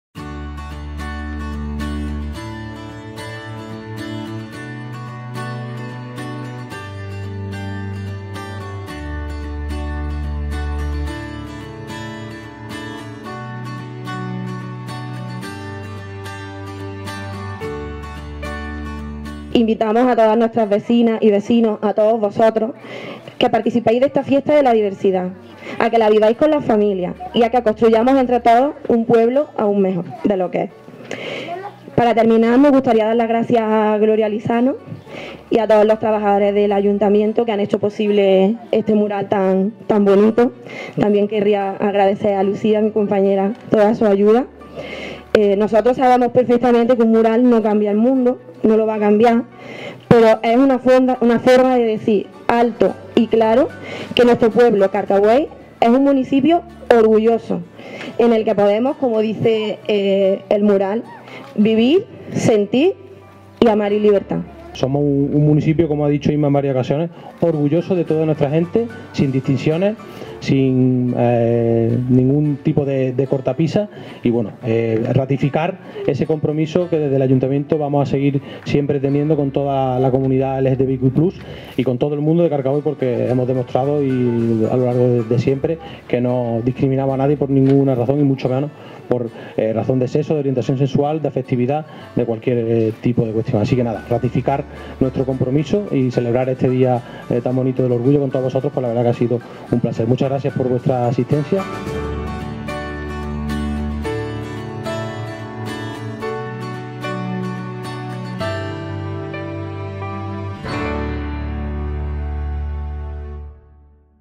El Ayuntamiento de Carcabuey inaugura un mural con motivo del Día Internacional del Orgullo LGTBIQ+
Carcabuey conmemoró este miércoles el Día Internacional del Orgullo LGTBIQ+ con la inauguración de un mural, obra de la artista Gloria Lizano, en el que se destaca el lema ‘Vive, siente y ama libre, Carcabuey orgulloso’ como muestra del compromiso de la localidad con la defensa de la diversidad. Así quedó de manifiesto en el acto inaugural, presidido por el alcalde, Juan Miguel Sánchez, y por la concejal de Igualdad, Diversidad y Mujer Rural, Inmaculada Marín, quienes se dirigieron a un numeroso público que quiso formar parte en la puesta de largo de esta instalación.